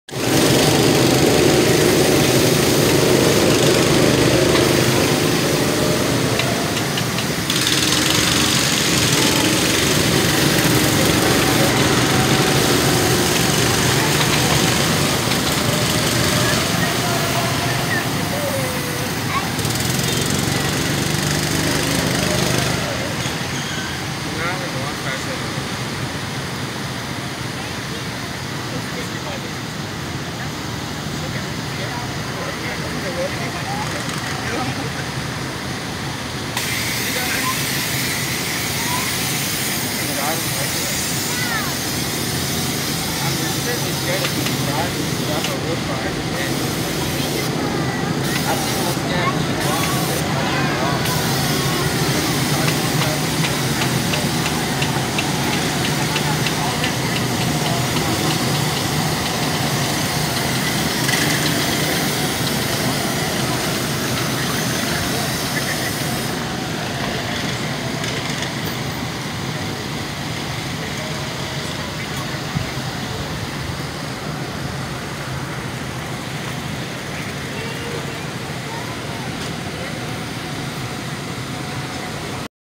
Birthday party
celebrating Jessie's birthday at Mountasia family fun center.